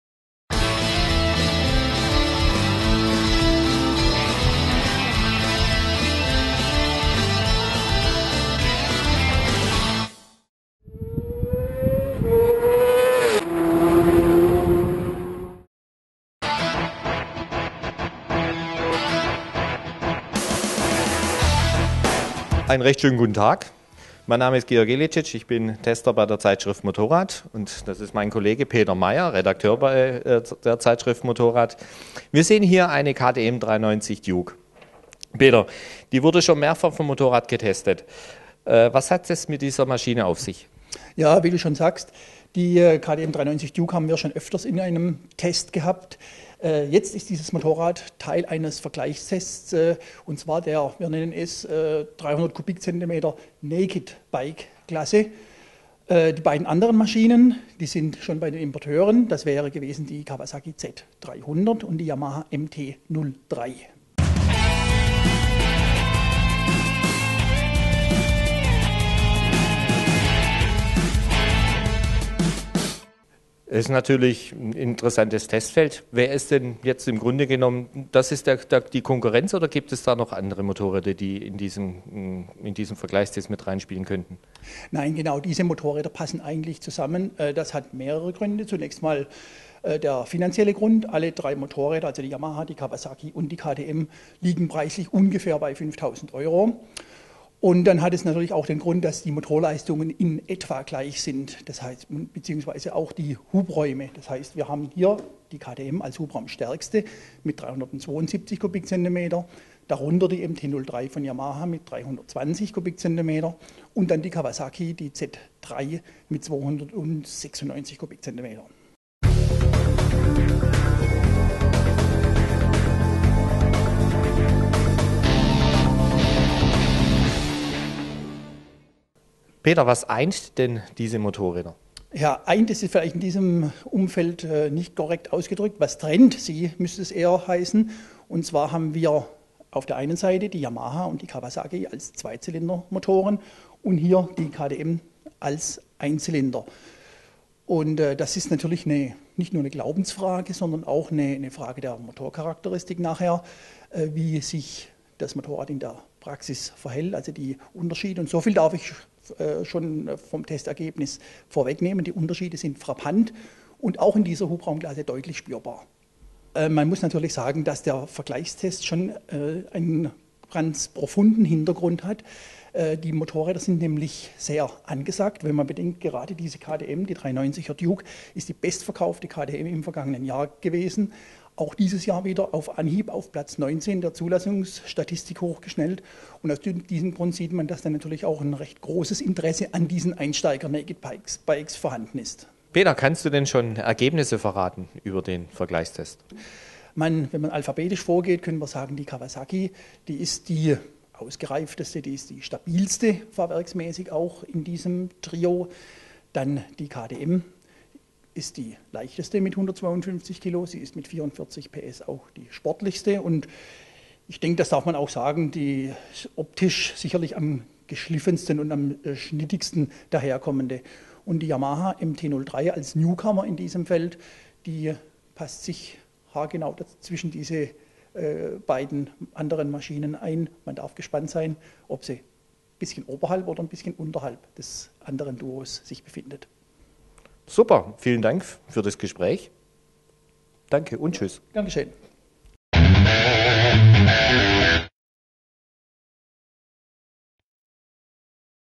Einsteiger-Naked-Bikes im Test: Yamaha MT-03, KTM 390 Duke und Kawasaki Z 300
Schneller, höher, weiter? Es geht auch anders. Die noch junge 300er-Klasse konterkariert den Leistungswahn mit pfiffig gestylten Naked Bikes zum moderaten Tarif. Und: Es gibt gute Gründe, weshalb diese quirligen Baby-Flitzer boomen. Hier gibt's den kompletten Test